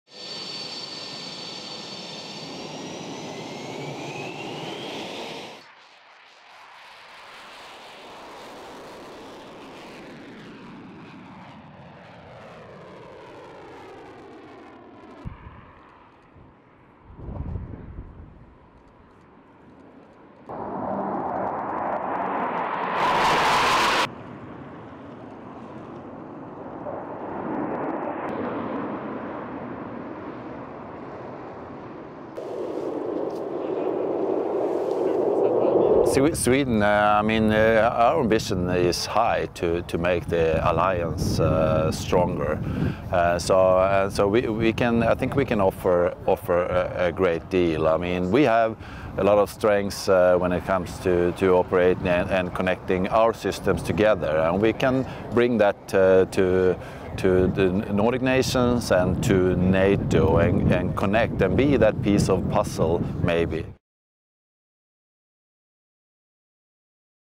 Swedish fighter jets join Nato military exercises for first time
Nato has begun large-scale drills in its newly expanded territory in northern Europe that for the first time involve Finland as a member of the western military alliance.

Sweden is expected to formally become the 32nd member of Nato this month after Hungary dropped its objections to its bid.

More than 20,000 soldiers from 13 countries are taking part in the Nordic Response 2024 military exercises, which are being held in the northern regions of Finland, Norway and Sweden and will last for two weeks.

The Norwegian-led drills involve 4,000 soldiers from Finland, which joined Nato last April and shares an 830-mile border with Russia. Sweden is expected to formally become the 32nd member of Nato this month after Hungary dropped its objections to its bid.